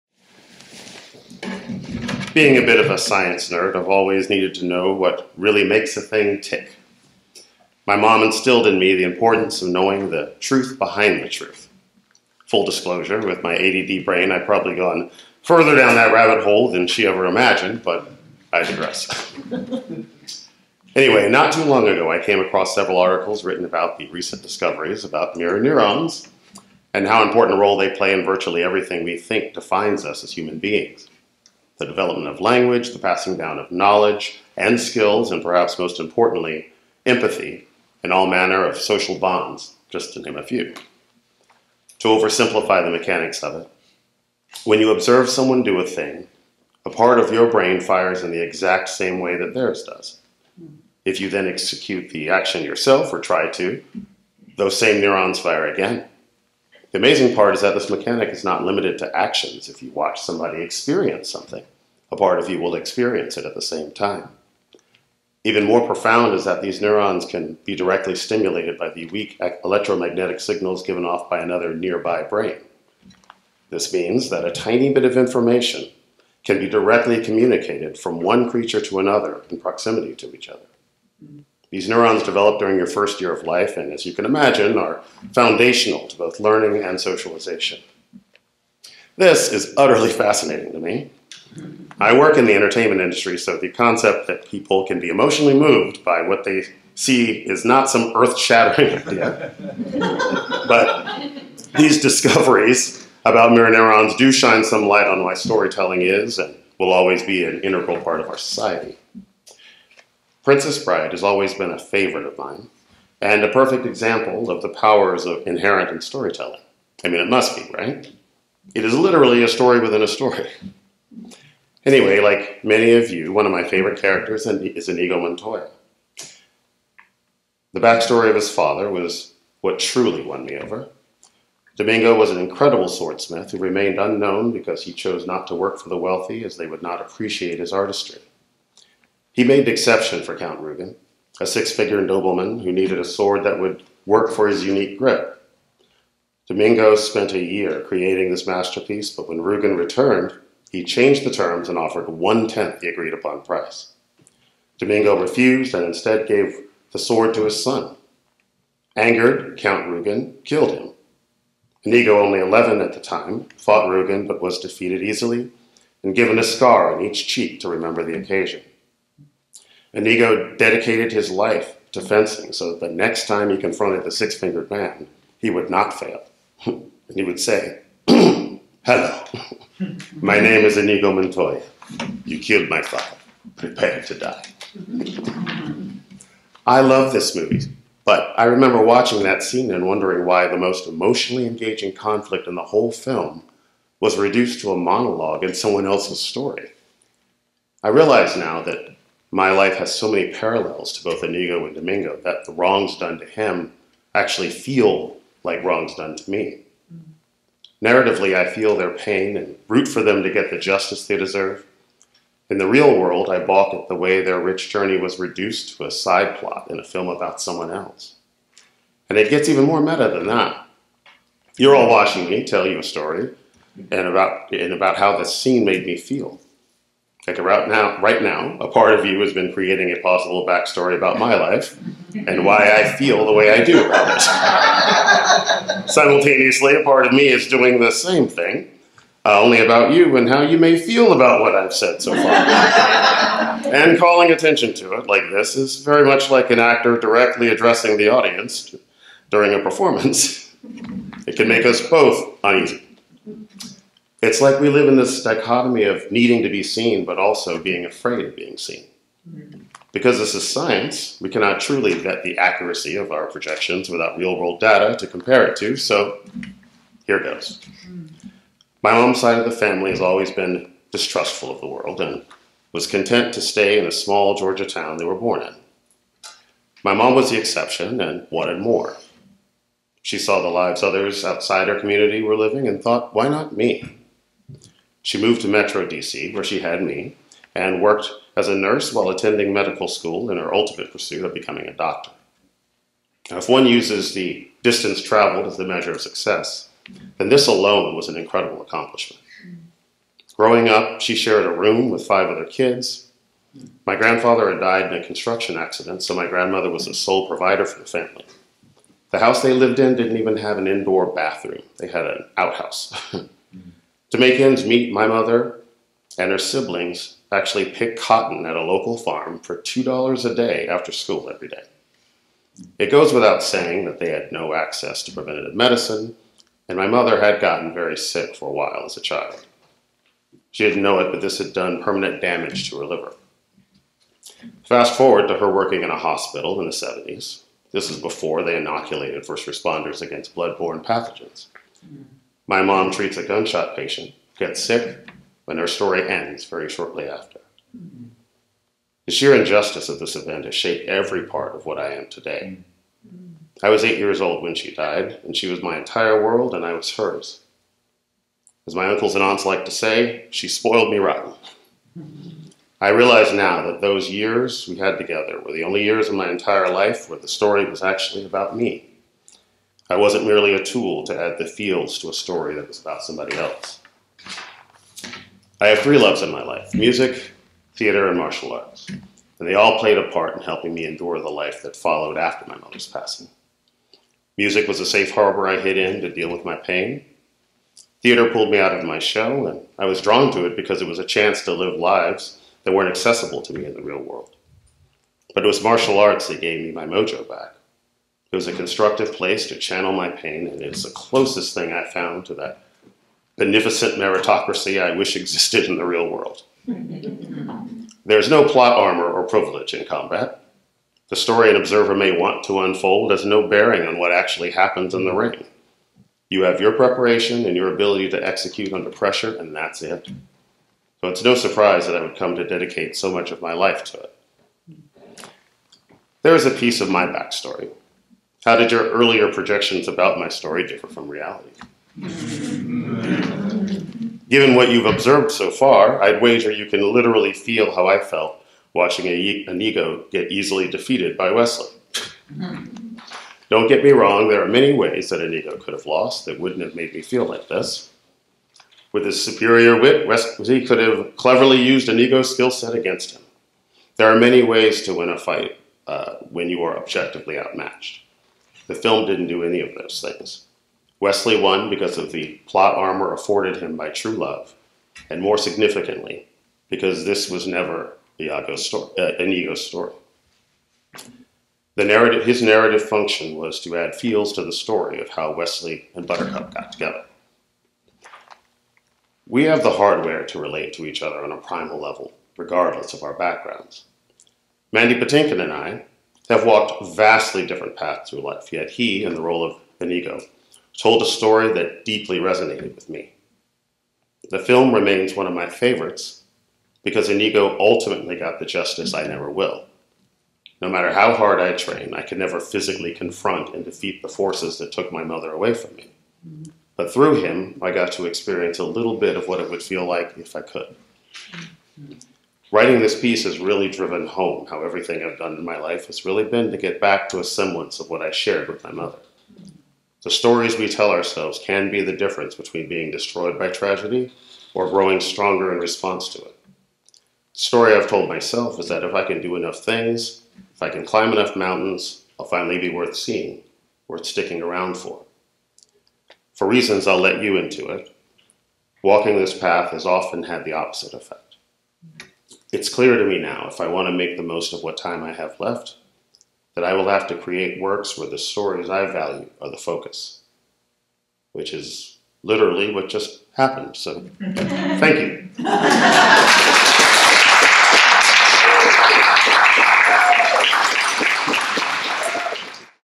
The Princess Bride: Marc Antonio Pritchett
The stories we tell ourselves can be the difference between being destroyed by tragedy or growing strong in response to it. Thus says Marc as he examines the loss of his own mother in the light of the story of Iñigo Montoya and the loss of his father.